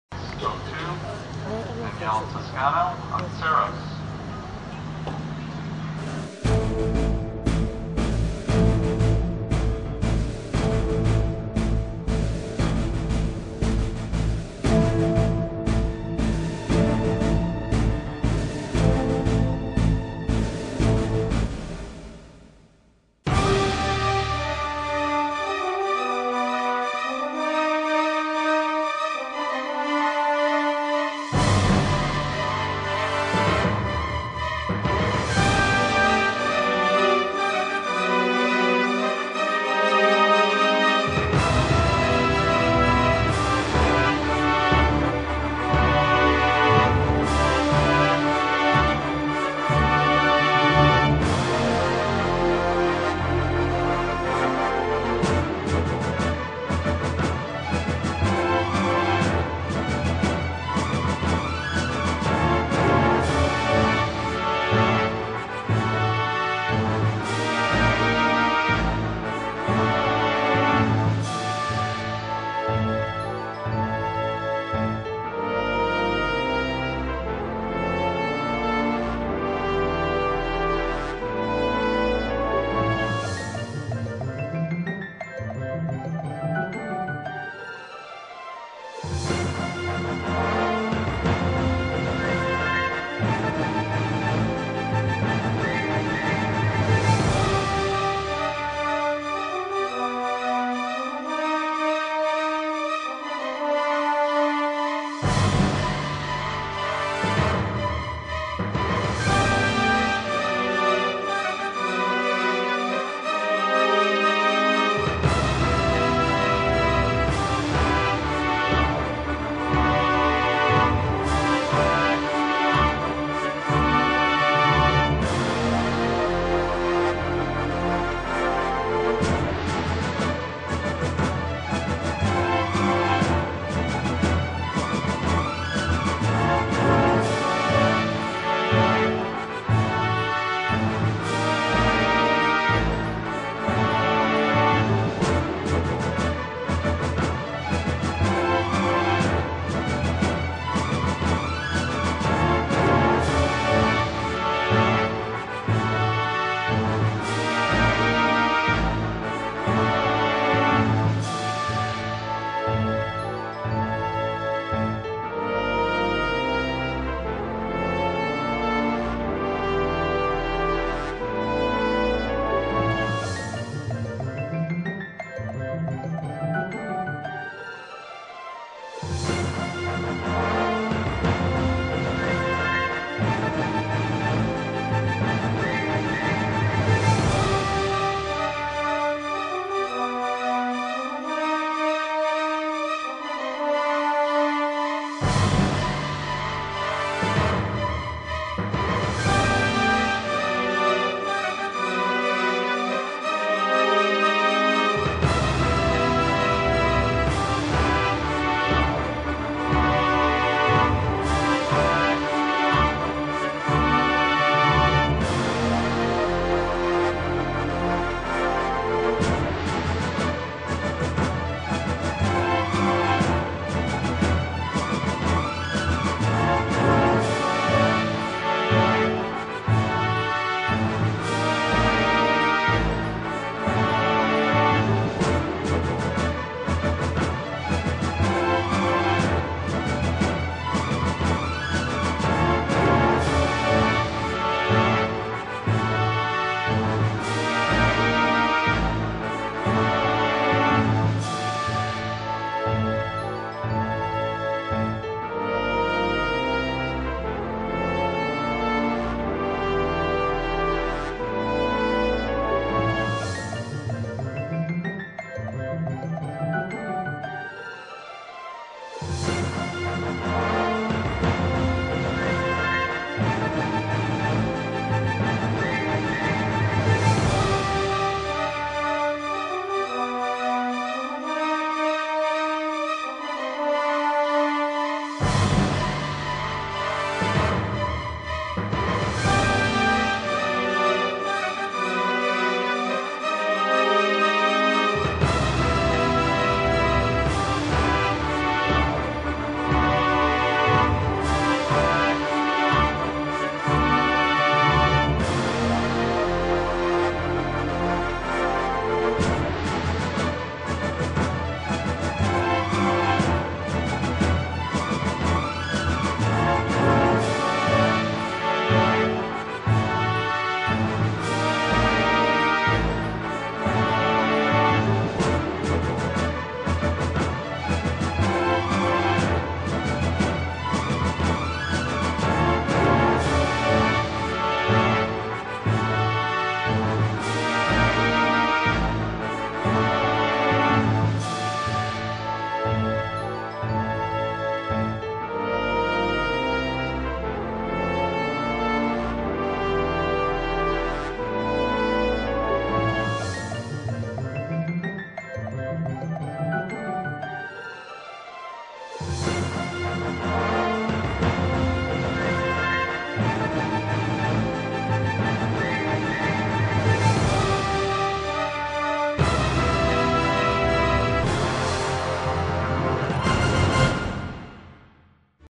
Ceros Dressage At Devon 2005
Ceros and Danni Toscano at Dressage at Devon 2005
Riding FEI Intermediaire